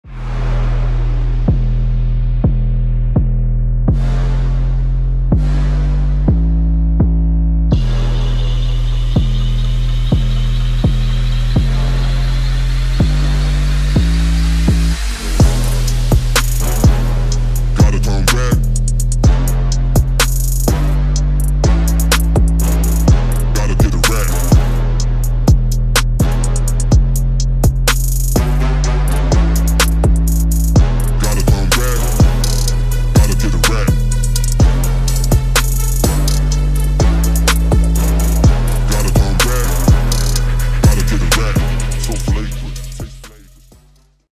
Harley Davidson 883 ‘02 and BMW G310R ‘21 Wife and Husband ride on 8/30/23
Ai :Created with the GoPro App on my iPhone.